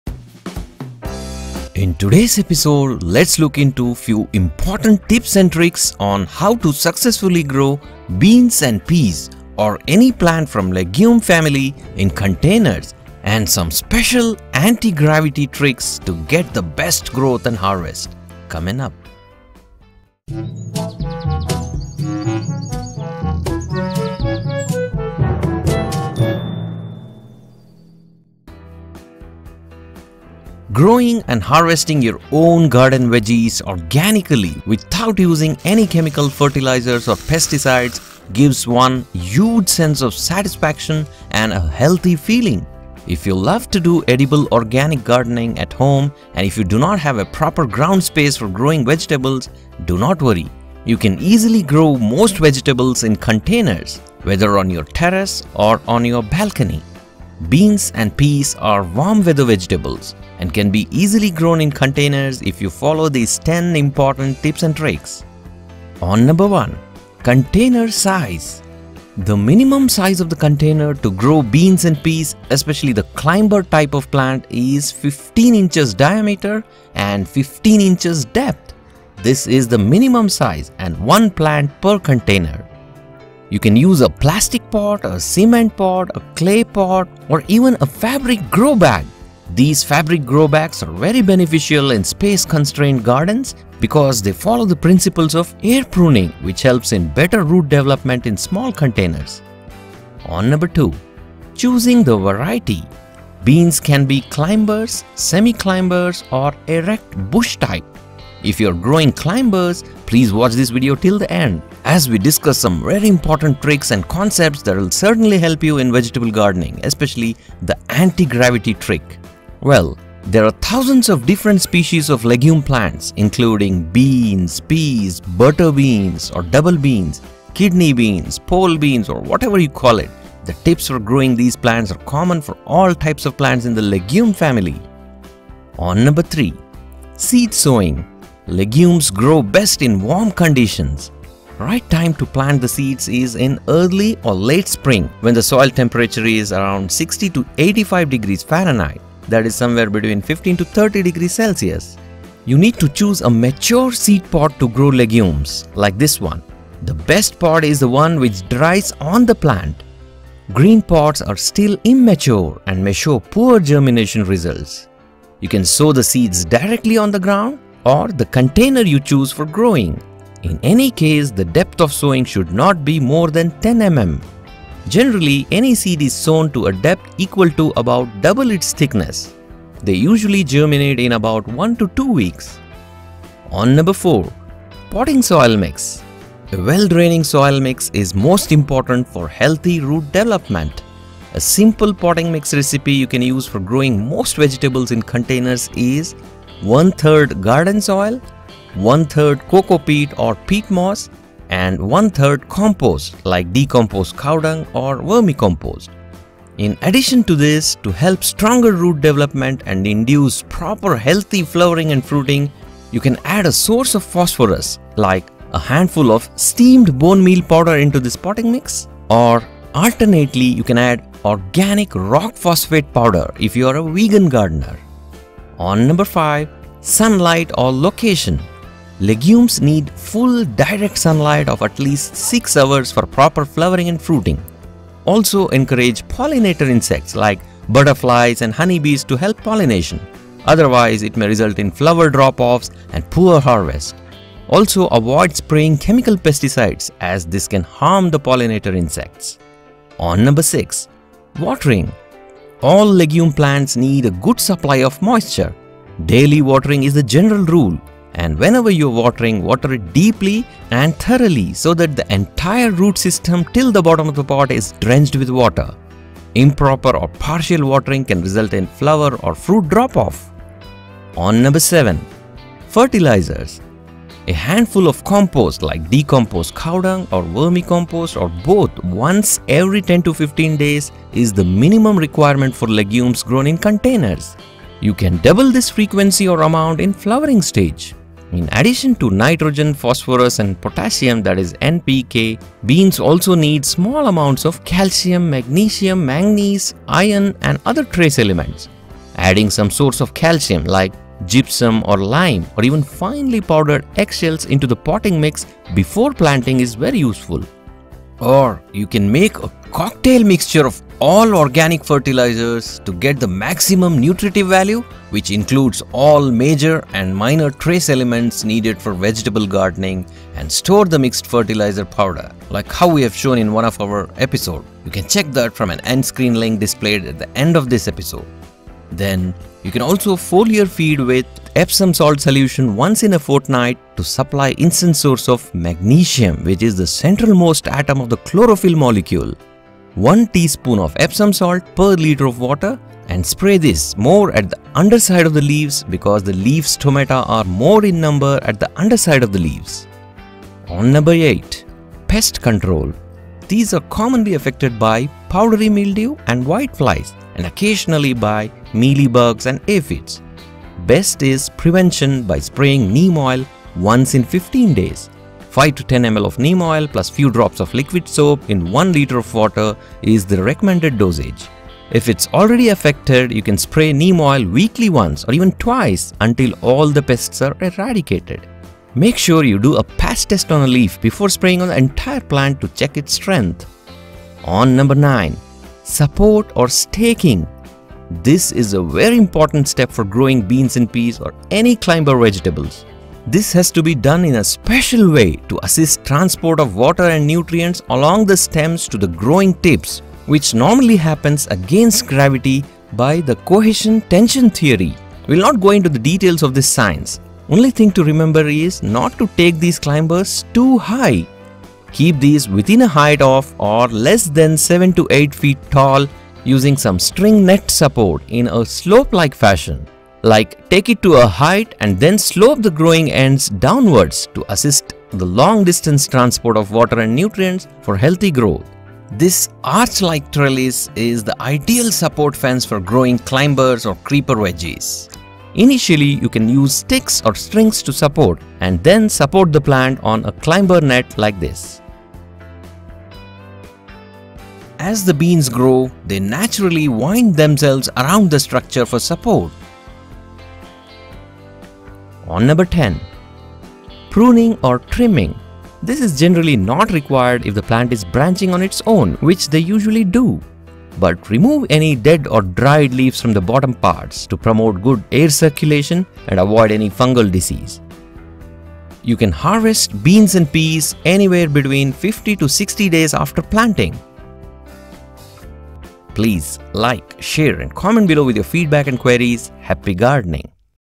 SECRETS TO GROW TONS OF ORGANIC BEANS AND PEAS IN POTS
No one will tell you these simple to follow organic gardening secrets, tricks and tips on how to grow Beans and Peas or any Legume in pots and get the best harvest.

Growing and harvesting your own garden veggies organically without using any chemical fertilizers or pesticides gives one huge sense of satisfaction and a healthy feeling. If you love to do edible organic gardening at home, and if you do not have a proper ground space for growing vegetables, do not worry, you can easily grow most vegetables in containers, including growing beans in pots, whether on your terrace or your balcony.

Beans and Peas are warm weather vegetables and can be easily grown in containers if you follow these 10 important garden secrets, tips and tricks.

TIME STAMPS (CHAPTERS):

1. CONTAINER SIZE FOR GROWING BEANS: 1:06
2. BEANS SEED VARIETIES:  1:40
3. SEED SOWING SEASON: 2:15
4. POTTING MIX FOR BEANS: 3:03
5. SUNLIGHT / LOCATION: 3:45
6. HOW TO WATER: 4:10
7. FERTILIZERS FOR BEANS & PEAS:  4:31
8. PEST CONTROL: 5:50
9. CLIMBER SUPPORT / STAKING: 6:24
10. PRUNING or TRIMMING: 7:31
11. HARVESTING BEANS: 7:48

Happy Gardening!

Happy Gardening!